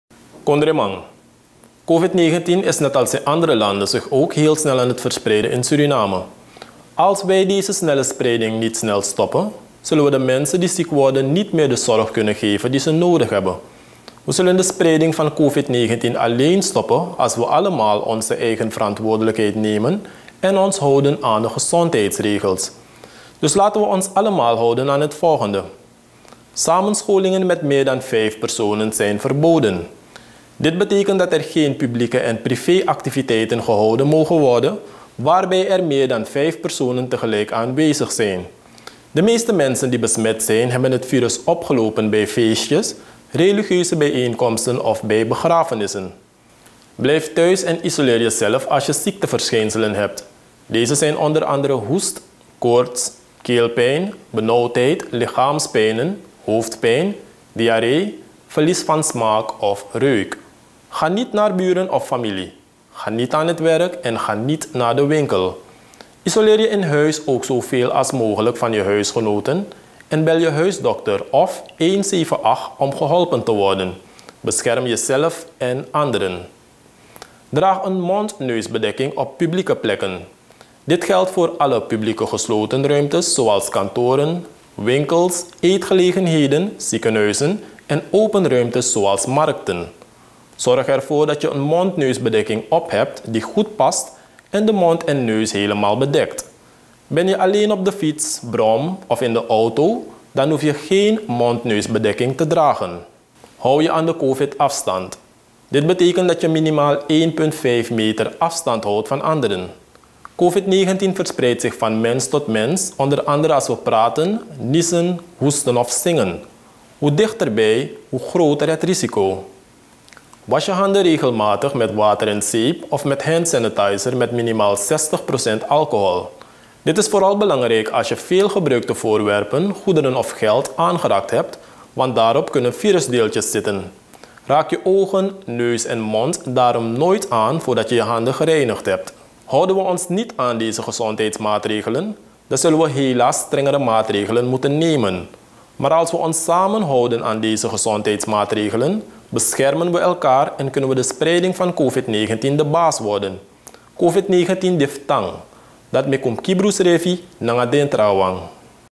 COVID-19 TIPS (NEDERLANDS)
In dit filmpje hoort u enkele covid-19 tips in het Nederlans